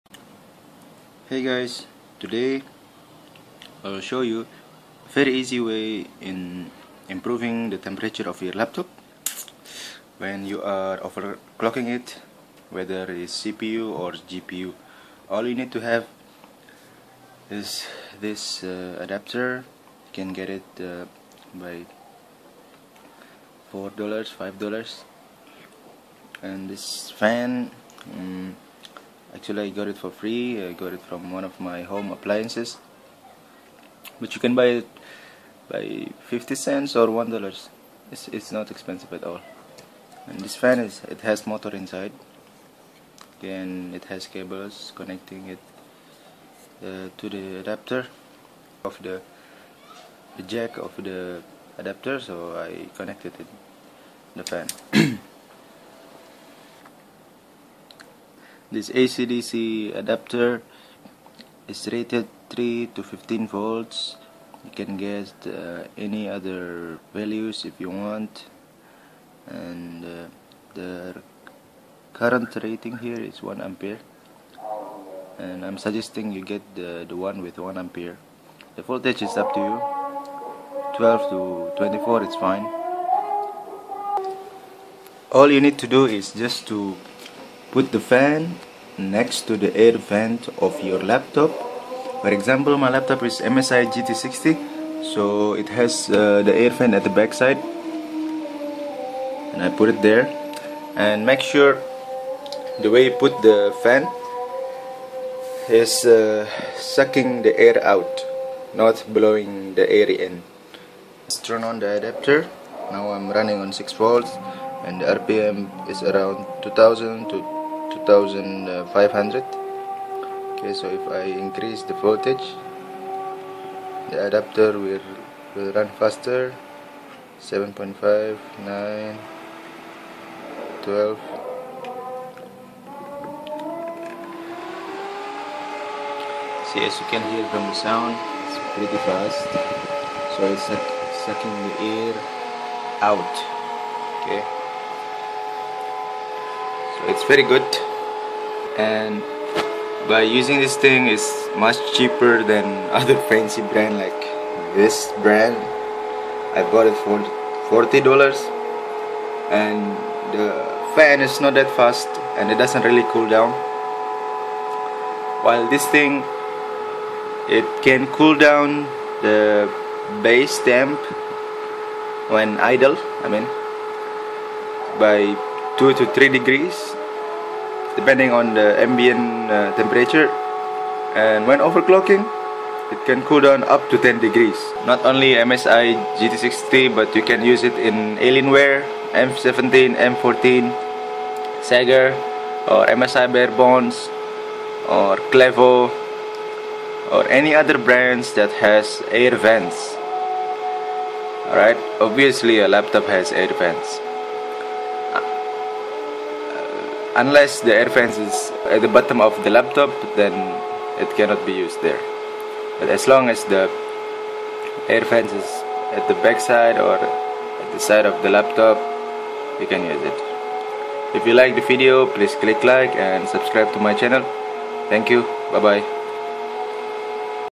how to cool down laptop
how to cool down your laptop cheaply and better than other fancy cooling pad
for example, chiller master?:P